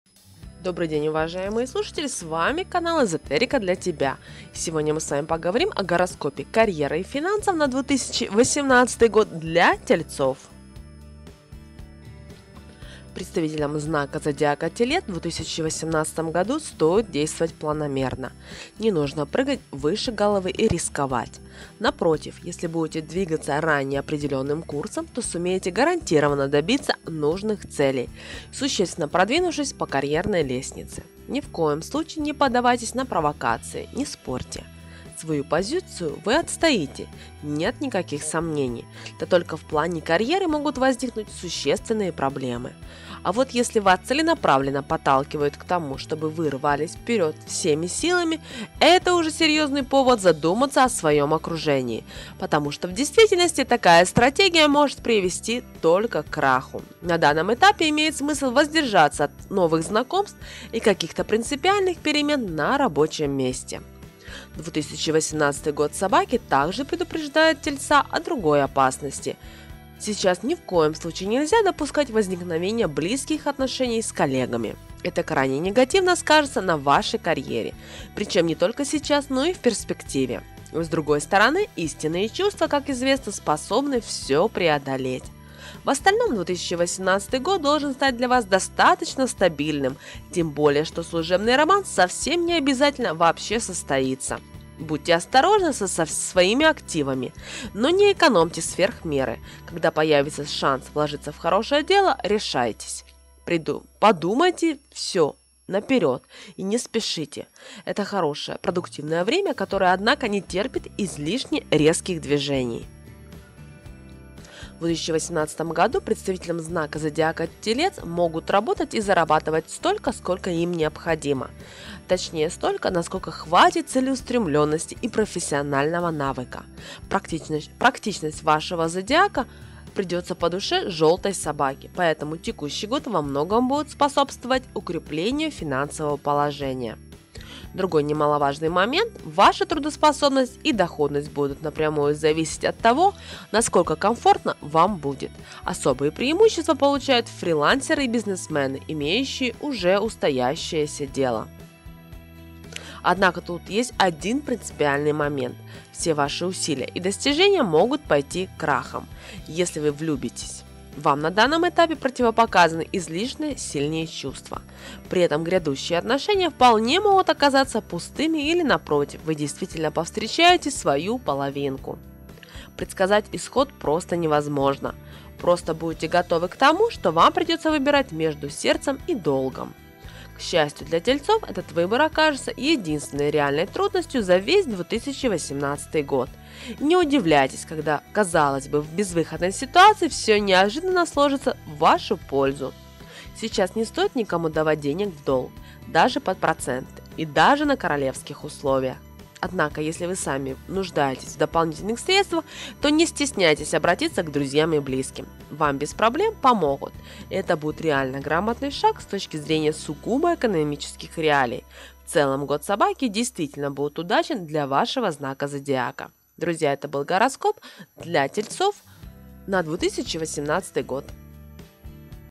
Гороскоп карьеры и финансов на 2018 год для знака зодиака -телец
Сфера карьеры вполне очевидно плотно соотносится со сферой финансов, однако это не всегда закономерно, и зачастую тенденции в этих направлениях могут развиваться диаметрально противоположно, особенно в год жёлтой собаки 2018, когда многие события окажутся реально непредсказуемыми.

В целом, это период успешный, хотя в плане карьерных достижений многое действительно будет зависеть от того, насколько хорошо тот или иной знак зодиака потрудился в предыдущем году. Для большинства 2018 год окажется не особенно хорош в плане начала собственного бизнеса. Многие знаки, за редким исключением, могут попасть в ситуацию, когда открытие своего дела принесёт больше минусов, чем плюсов, причем в глобальном смысле. В этом плане к этому году нужно относиться крайне осторожно.

С другой стороны, в 2018 году действительно возможно получать удовлетворение от продвижения по карьерной лестнице. Постараться однозначно придётся, но вряд ли будет иметь место откровенное перенапряжение. Многие могут оказаться перед выбором: карьера или другая карьера. Тут неверного варианта не будет, вам просто придётся что-то решить, отказаться от одного удобного и комфортного места в пользу другого. Угнаться за двумя зайцами однозначно не получится.